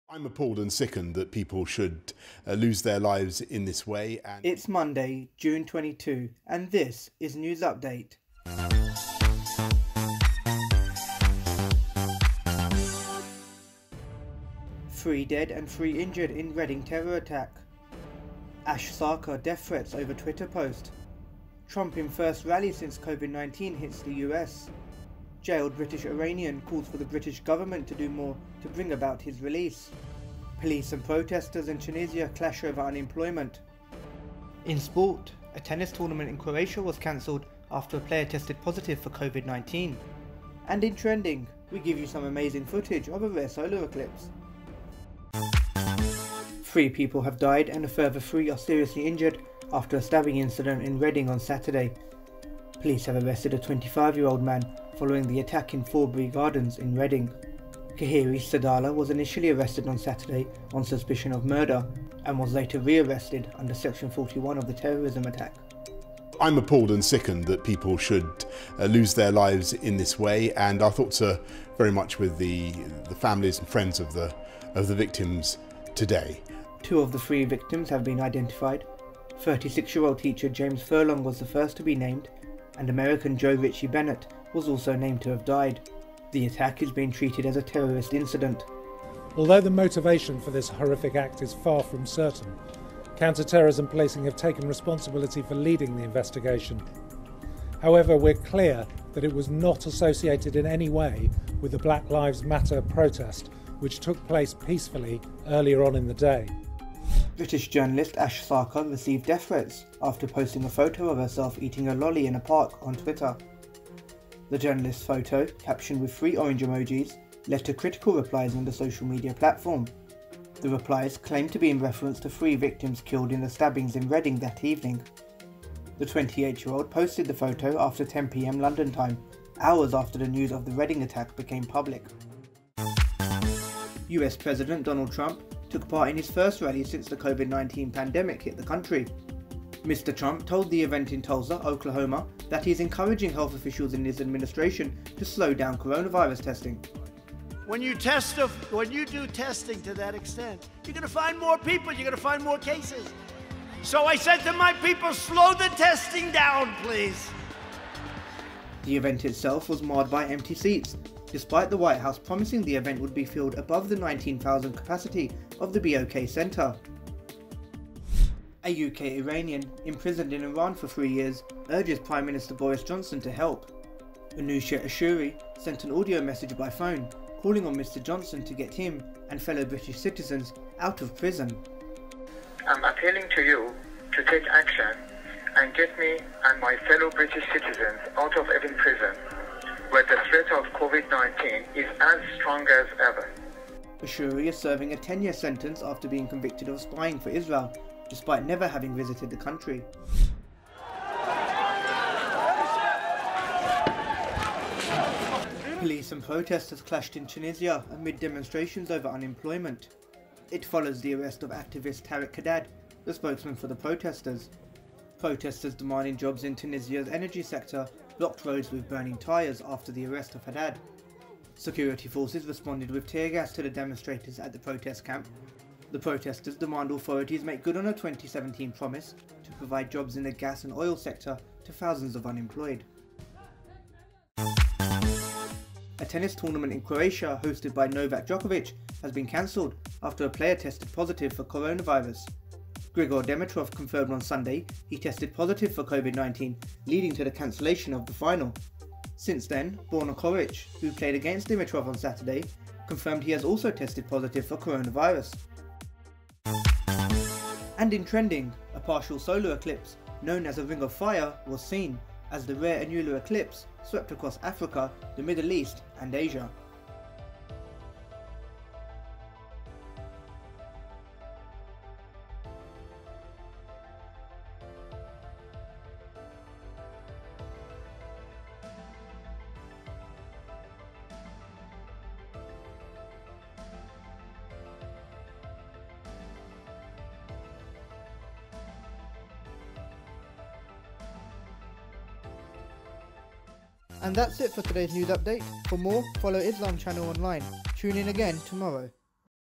News Update | June 22: Reading stabbings, Ash Sarkar, Donald Trump's rally and Solar Eclipse
𝗔𝗹𝗹 𝘁𝗵𝗲 𝗹𝗮𝘁𝗲𝘀𝘁 𝗻𝗲𝘄𝘀 𝗳𝗿𝗼𝗺 𝘁𝗵𝗲 𝗨𝗞 𝗮𝗻𝗱 𝗯𝗲𝘆𝗼𝗻𝗱 𝗳𝗿𝗼𝗺 𝗜𝘀𝗹𝗮𝗺 𝗖𝗵𝗮𝗻𝗻𝗲𝗹

Three people have died, and a further three are seriously injured after a stabbing incident in Reading on Saturday. Police have arrested a 25-year-old man following the attack in Forbury Gardens in Reading. Khairi Saadallah was initially arrested on Saturday on suspicion of murder, and was later re-arrested under Section 41 of the Terrorism Act 2000. Two of the three victims have been identified. 36-year-old teacher James Furlong was the first to be named, and American Joe Ritche-Bennet was also named to have died. The attack is being treated as a "terrorist incident"

British journalist Ash Sarkar received death threats, after posting a photo of herself eating a lolly in a park on Twitter. The journalist's photo, captioned with three orange emojis, led to critical replies on the social media platform. The replies claim to be in reference to three victims killed in the stabbings in Reading that evening. The 28-year-old posted the photo after 10pm London time, hours after the news of the Reading attack became public.

Police and protesters clashed in Tunisia, amid demonstrations over unemployment. It follows the arrest of activist Tarek Haddad, the spokesman for the protestors. Protesters demanding jobs in Tunisia's energy sector blocked roads with burning tyres, after the arrest of Haddad. Security forces responded with tear gas to the demonstrators at the protest camp, in the southern Tataouine region. The protests demand authorities make good on a 2017 promise, to provide jobs in the gas and oil sector to thousands of unemployed

A UK-Iranian, imprisoned in Iran for three years, urges Prime Minister Boris Johnson to help. Anoosheh Ashoori sent an audio message by phone, calling on Mr Johnson to get him, and fellow British citizens, out of the Evin prison. Ashoori is serving a 10-year sentence after being convicted of spying for Israel, despite never having visited the country.

US President Donald Trump took part in his first rally since the COVID-19 pandemic hit the country. Mr Trump told the event in Tulsa, Oklahoma, that he's encouraging health officials in his administration to slow down coronavirus testing. The event itself was marred by empty seats, despite The White House promising the event would be filled above the 19,000 capacity of the BOK Center.

In sport, a tennis tournament in Croatia hosted by Novak Djokovic has been cancelled, after a player tested positive for Coronavirus. Grigor Dimitrov confirmed on Sunday he tested positive for COVID-19, leading to the cancellation of final. Since then, Borna Coric - who played against Dimitrov on Saturday - confirmed he has also tested positive for coronavirus

And in trending... a partial solar eclipse, known as a 'ring of fire', was seen, as the rare annular eclipse, swept across Africa, the Middle East and Asia.